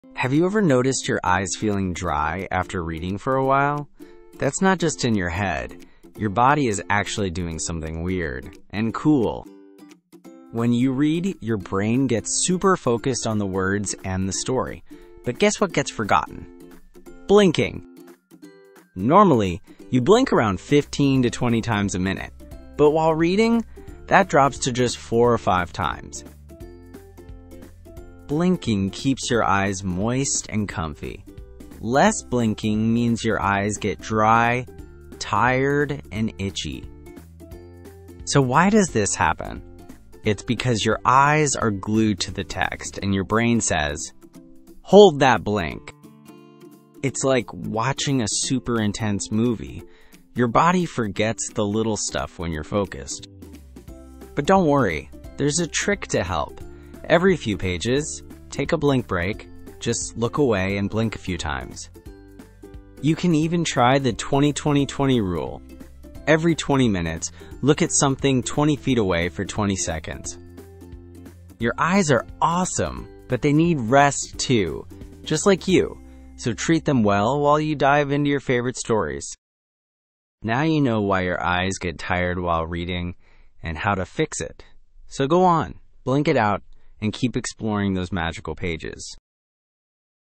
Why You Forget to Blink When You Read! | DO you Know|Kids
Did you know your eyes blink way less when youre reading? It’s because your brain is so focused, it forgets to tell your eyes to blink! That’s why they can feel dry or tired after a long chapter. Ever notice your eyes getting itchy after reading? ~shorts